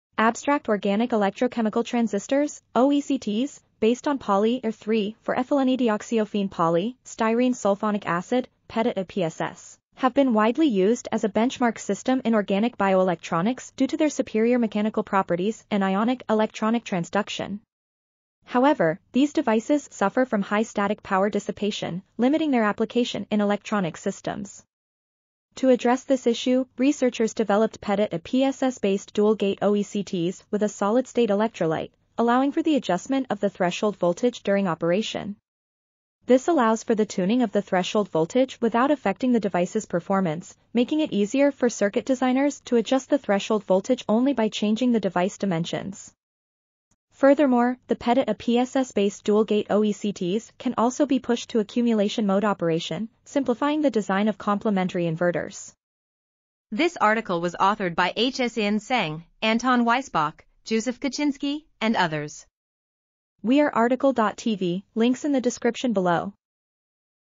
Threshold Voltage Control in Dual‐Gate Organic Electrochemical Transistors | RTCL.TV
### Keywords ###

### Article Attribution ###
Title: Threshold Voltage Control in Dual‐Gate Organic Electrochemical Transistors
Authors: Hsin Tseng, Anton Weissbach, Juzef Kucinski, Ali Solgi, Rakesh Nair, Lukas M. Bongartz, Giuseppe Ciccone, Matteo Cucchi, Karl Leo ,and Hans Kleemann
Publisher: Wiley-VCH
DOI: 10.1002/admi.202201914

### Video Timestamps ###
0:00:00 - Summary
0:00:56 - Title
0:01:03 - Outro
0:01:07 - End